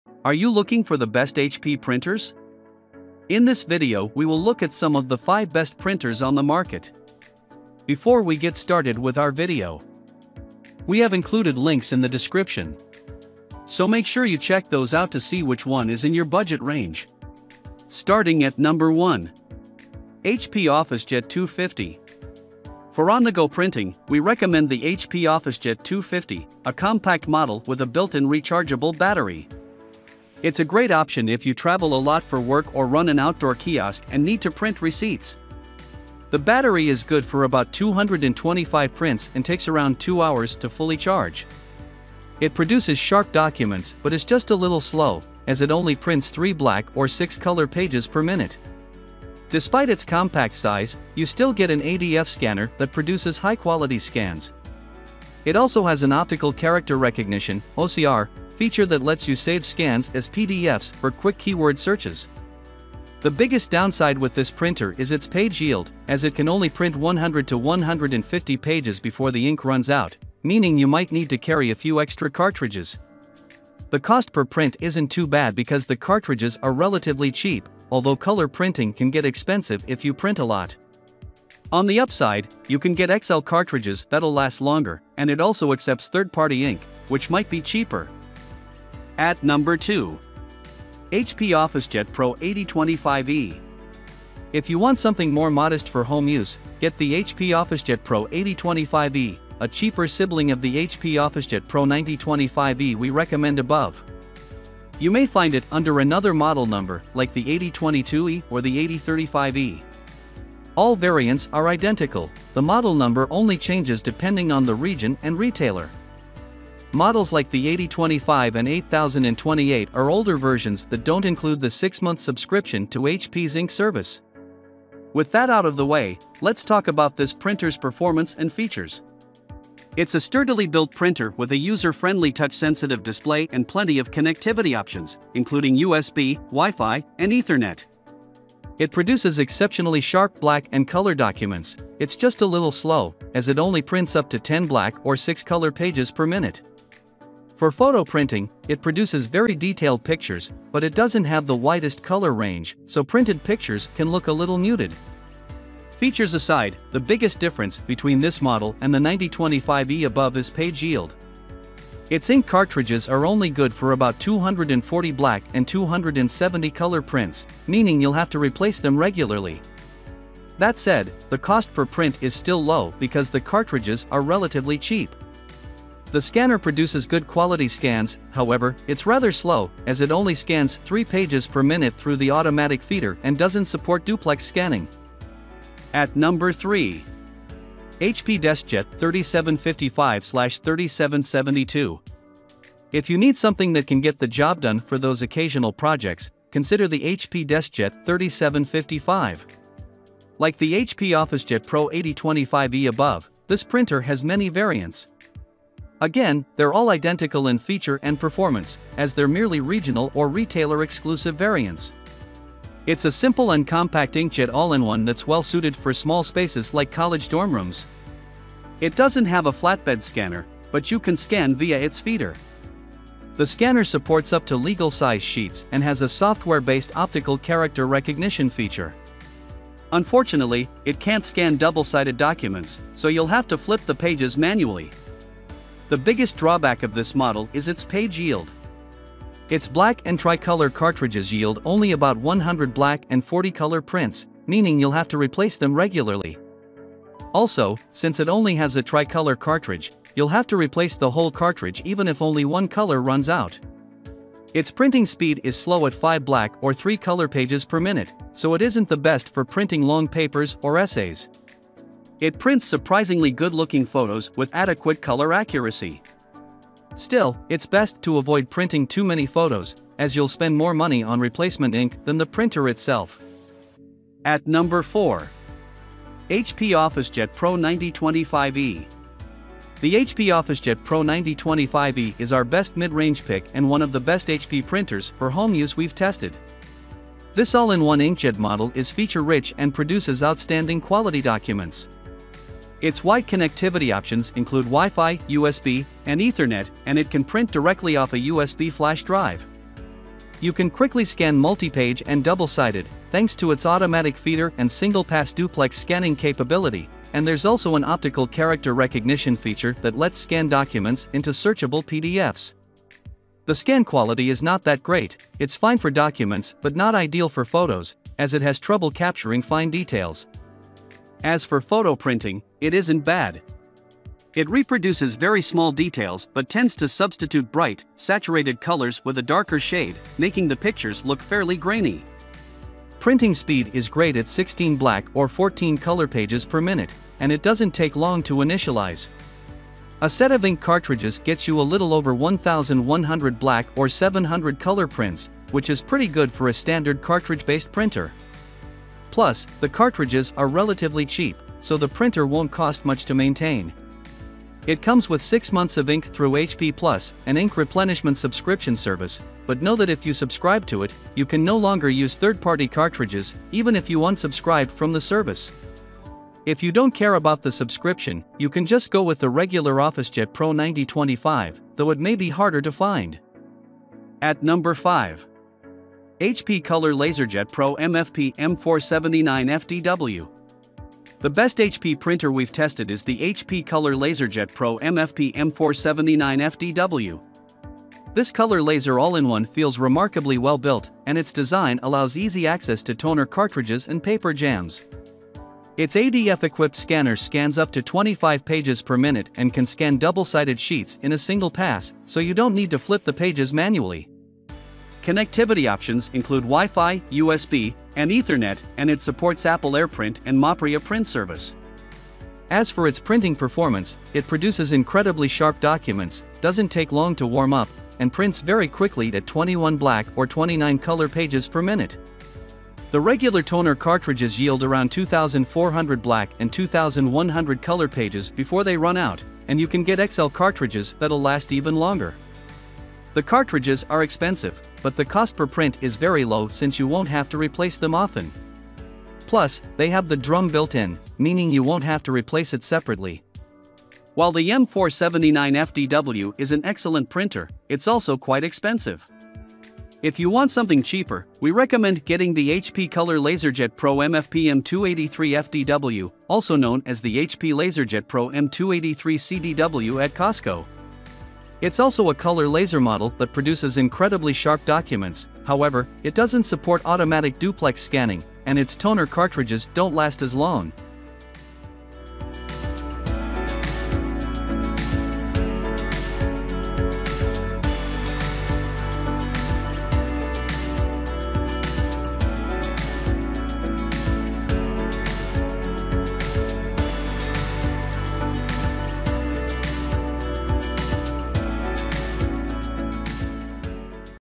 ✅Top 5: Best HP Printers in 2023 || The Best HP Printers Buying Guide [Reviews]
1. HP OfficeJet 250.

2. HP OfficeJet Pro 8025e.

3. HP DeskJet 3755/3772.

4. HP OfficeJet Pro 9025e.

5. HP Color LaserJet Pro MFP M479fdw.

In this video, we listed the Best HP Printers